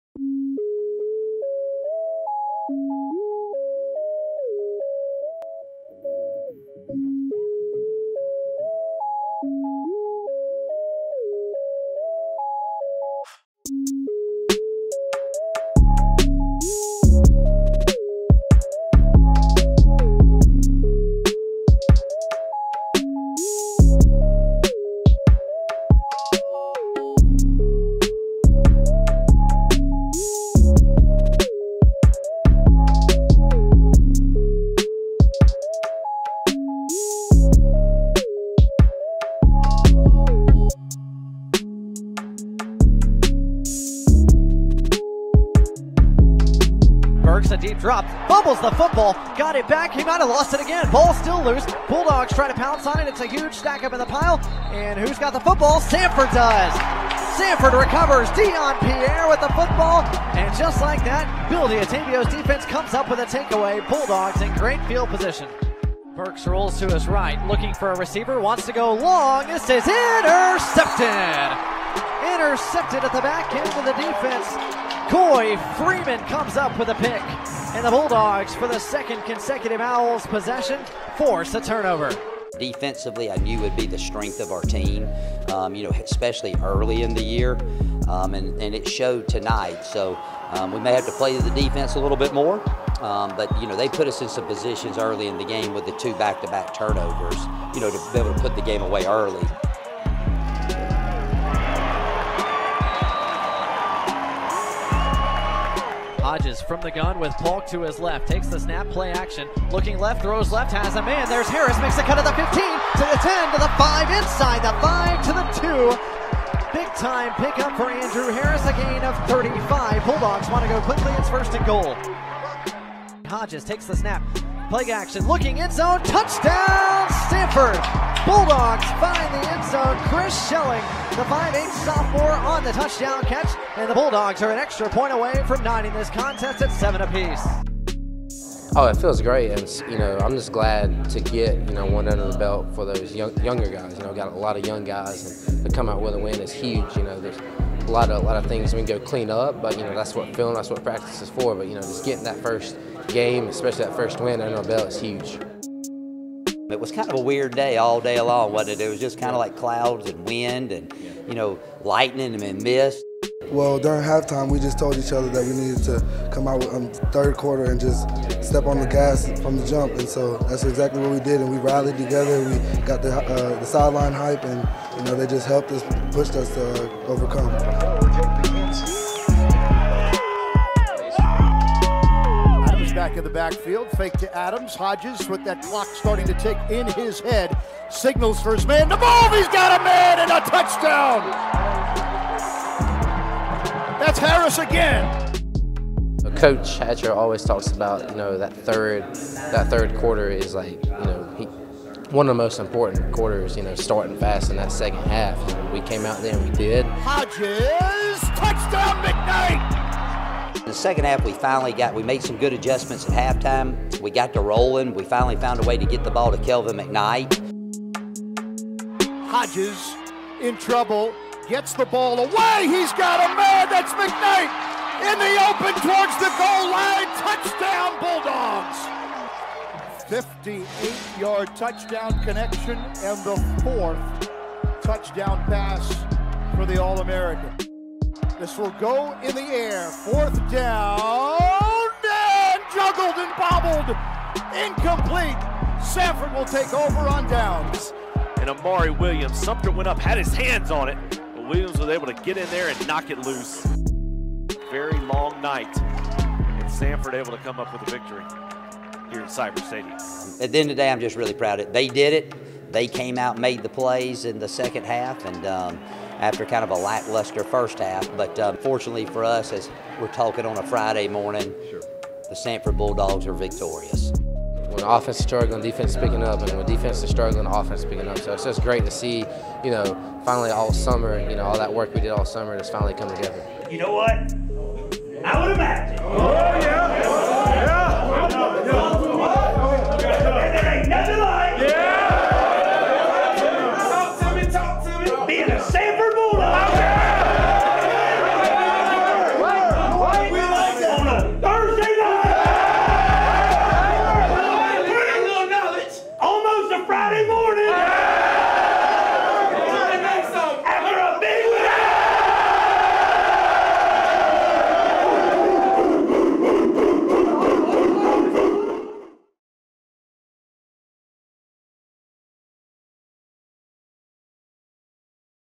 FB: Samford 28 Kennesaw State 23, Game Recap
Samford's football team battled to a 28-23 season-opening win over Kennesaw State.